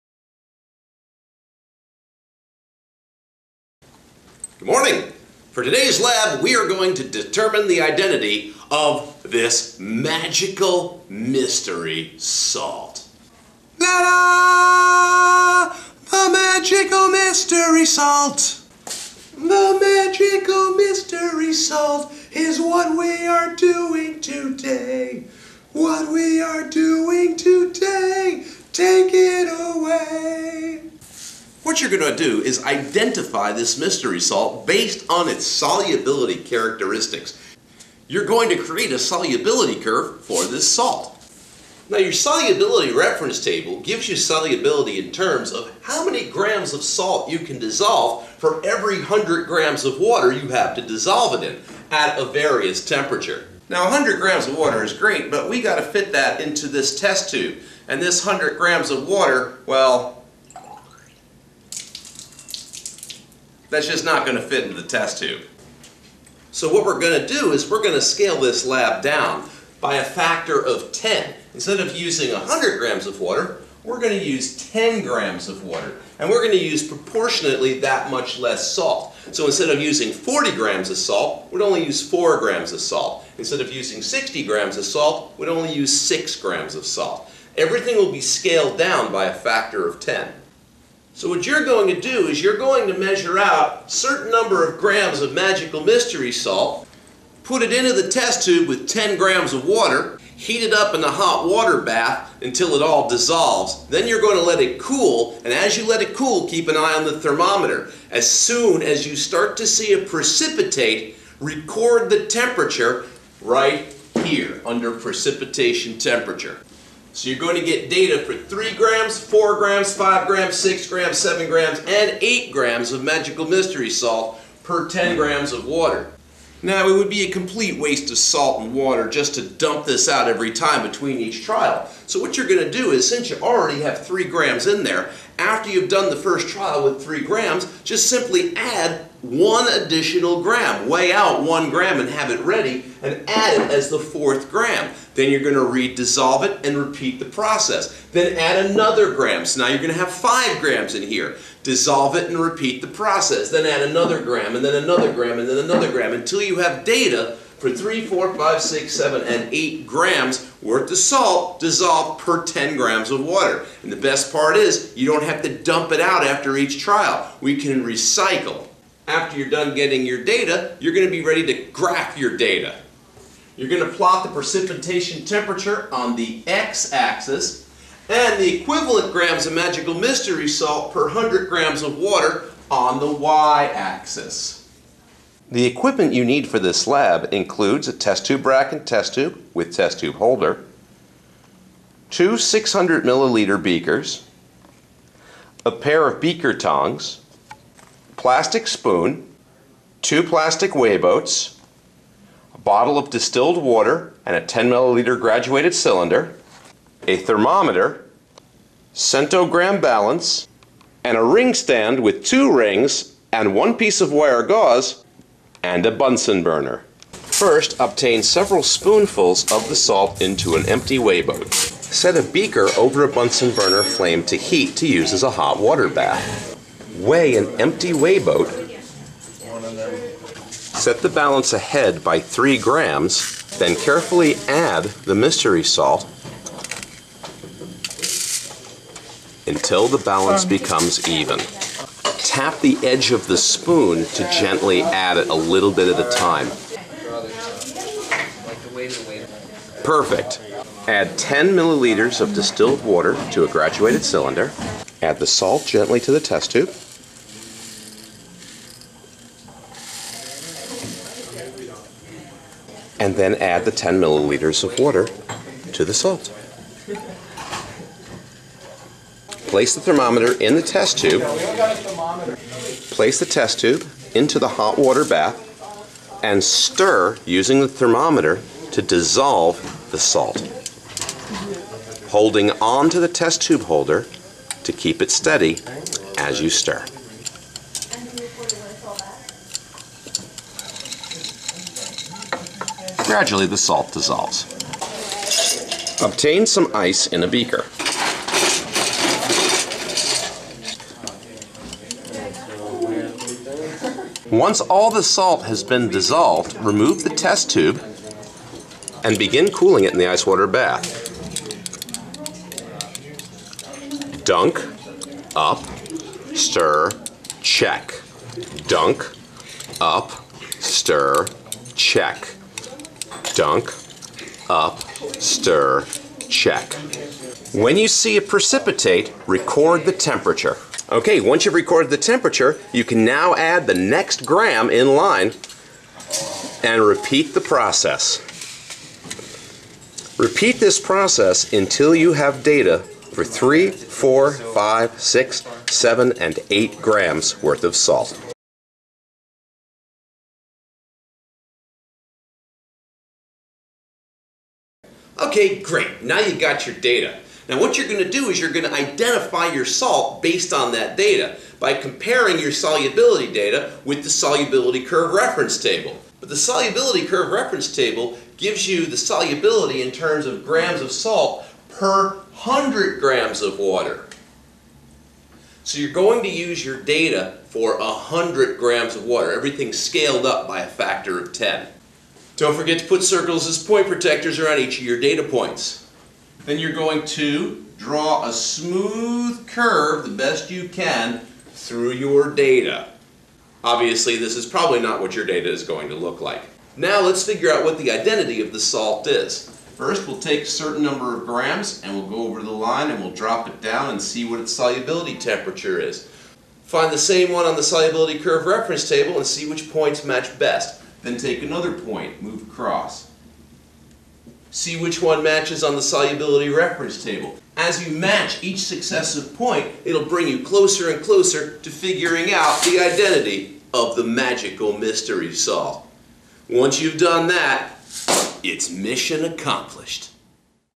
Chem Lab: Solubility Of A Salt
In this lab, you will use solubility data to identify an ionic compound.